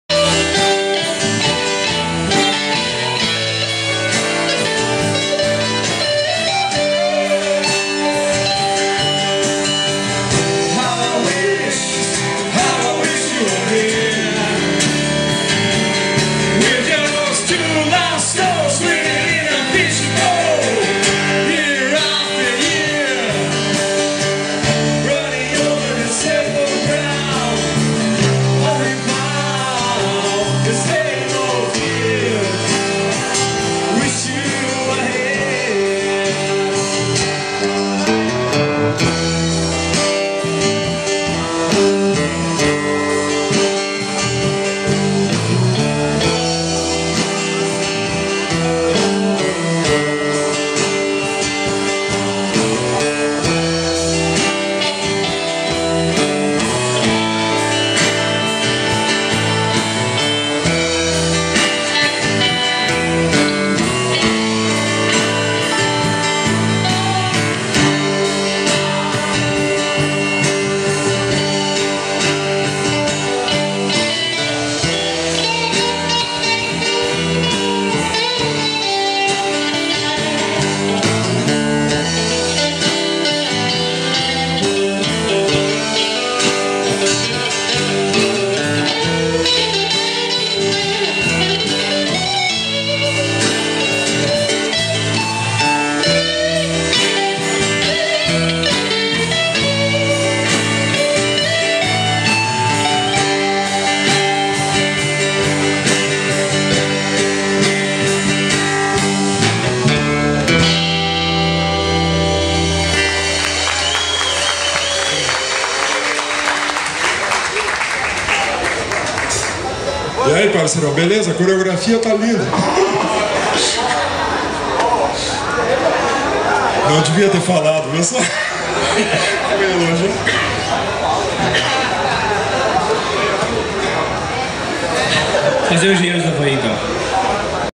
Tulasi toca na Feira do Livro (03)
Banda de rock Tulasi se apresentou na Feira do Livro de Cachoeira do Sul (RS) em 09 de outubro de 2011.